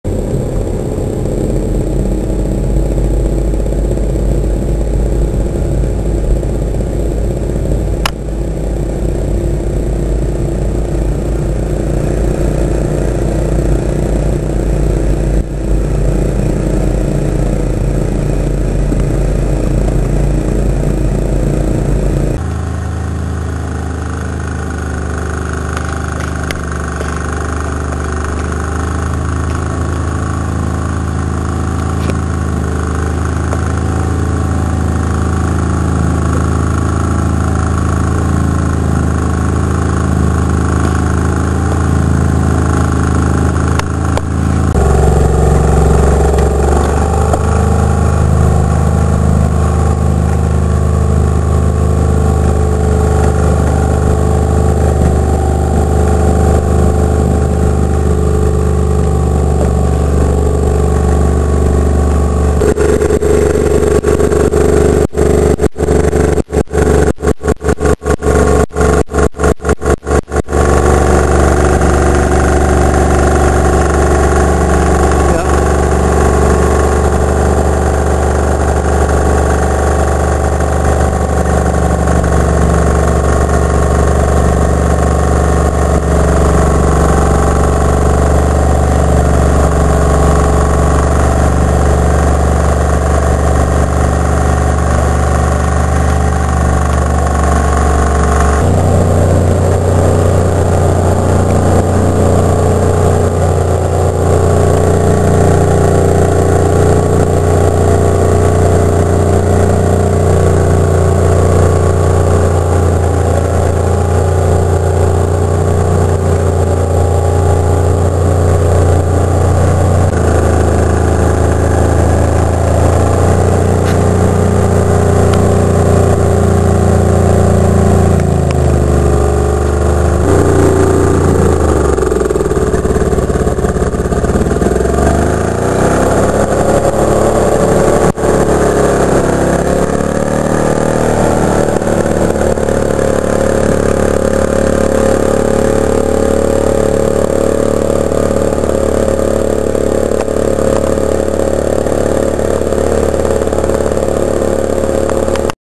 Mit dem Fahr KT Holz rücken
Hier sind wir mit dem Fahr KT 10D 5 beim Holzrücken.
Es ist ein Zusammenschnitt mehrerer Videos, die Mitte ist qualitativ nicht gut gelungen.